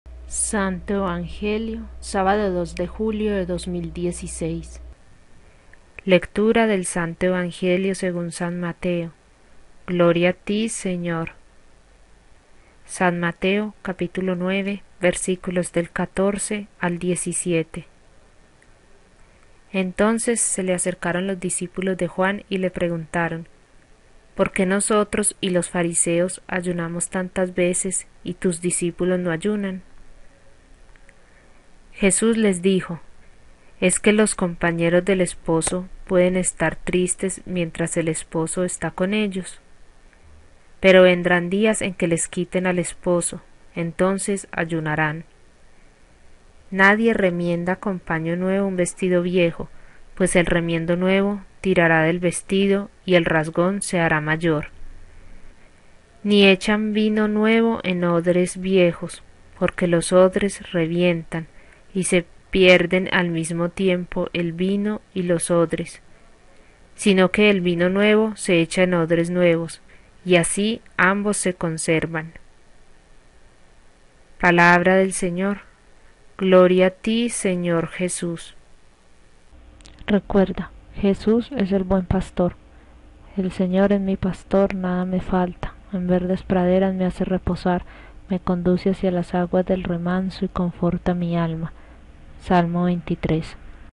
SANTO EVANGELIO SABADO 2 DE JULIO DE 2016
REFLEXIÓN: "Señor Jesús, tú que alegras nuestro corazón con tu presencia llena de amor y misericordia, concédenos alegrar con nuestras palabras y acciones a todos aquellos que necesitan salir de sus tristezas y vivir en la alegría que brota de ti".

SANTO DEL DIA: San Bernardino Realino, presbítero.